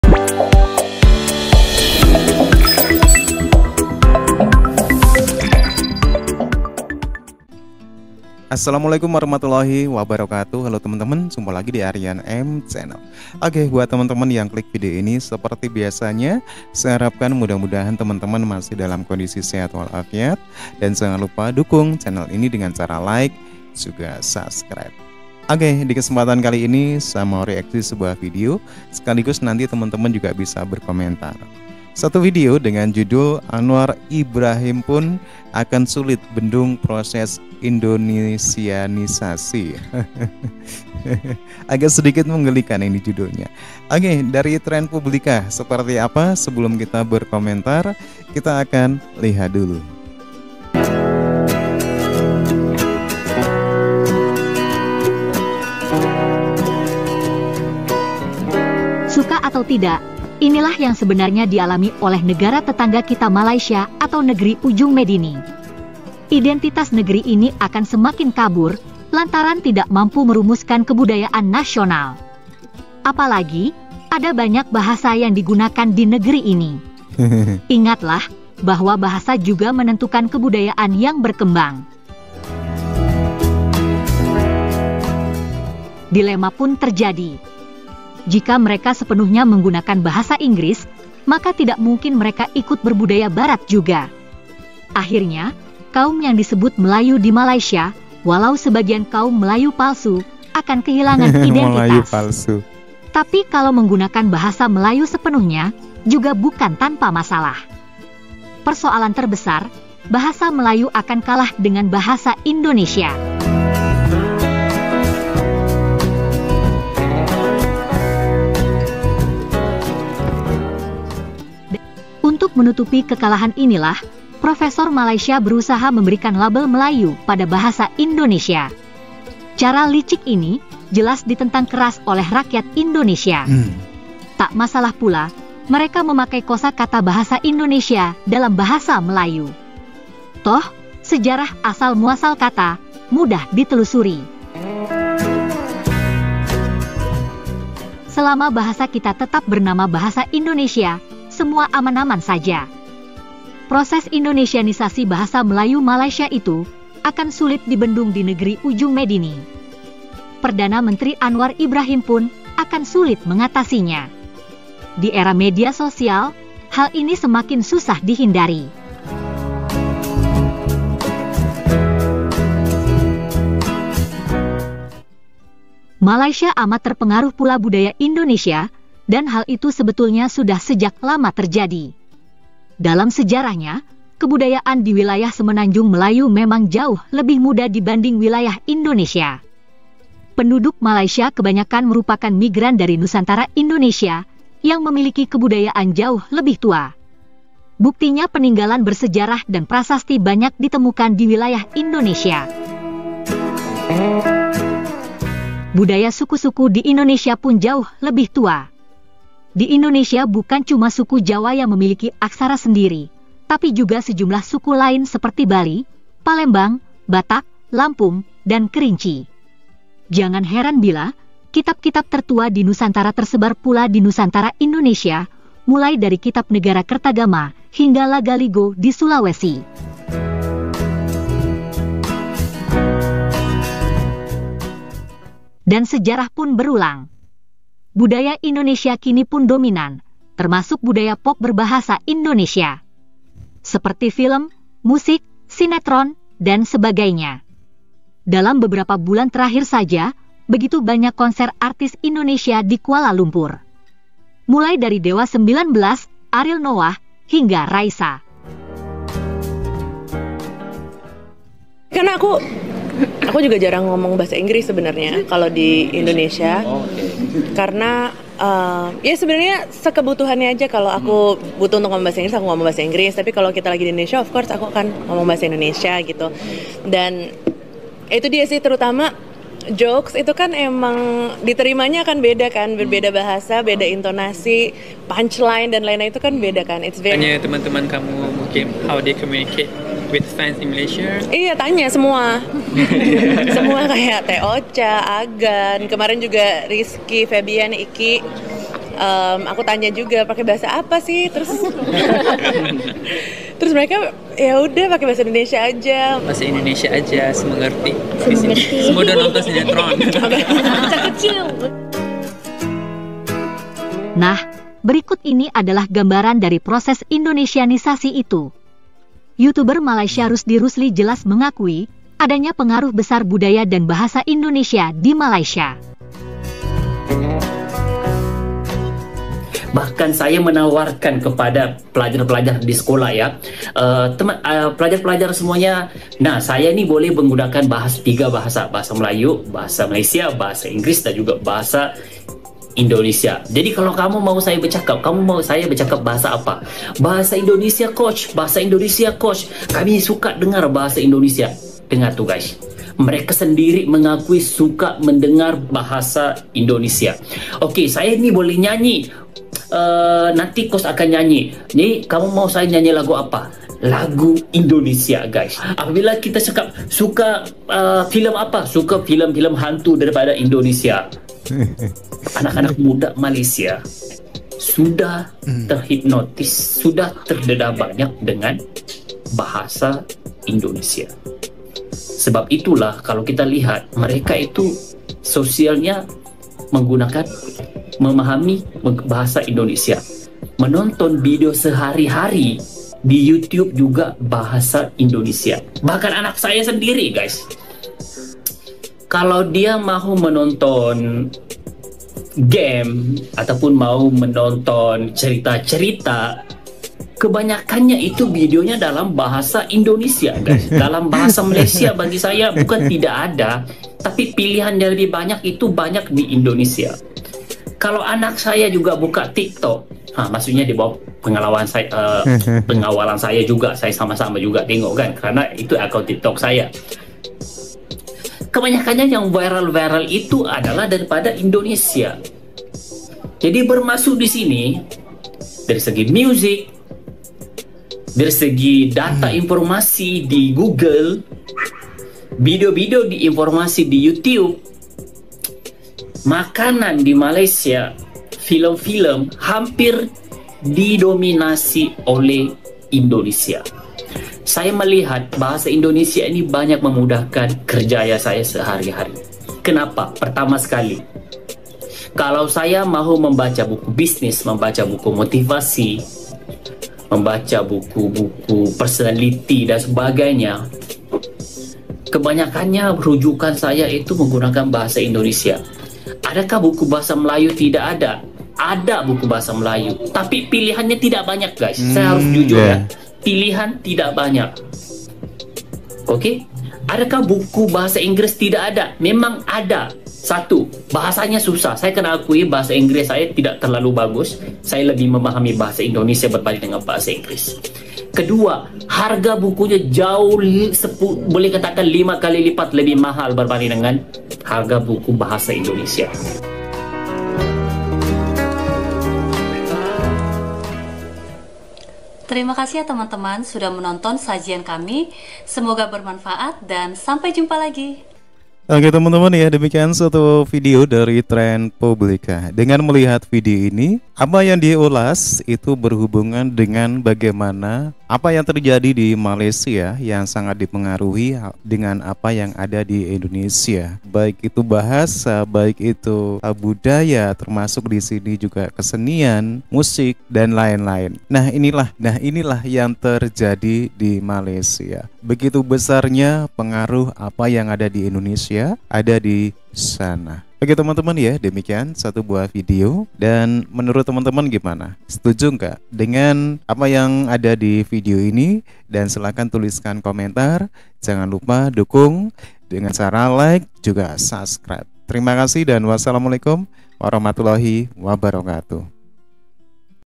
NGAKAK PM MALAYSIA SULIT BENDUNG INDONESIANISASI DI MALAYSIA
AHLI ETNIS MALAYSIA AKUI KEHEBATAN INDONESIA YANG BISA SATUKAN 1500 SUKU
ramai ramai media malaysia soroti pemindahan ibukota ke ikn dan pembangunan ikn
indonesia sudah bangun nasionalisme ekonomi,malaysia masih sibuk cari identitas negara belum wujud
BAHASA MELAYU AKAN HILANG SEBAGAI BAHASA NASIONAL DI MALAYSIA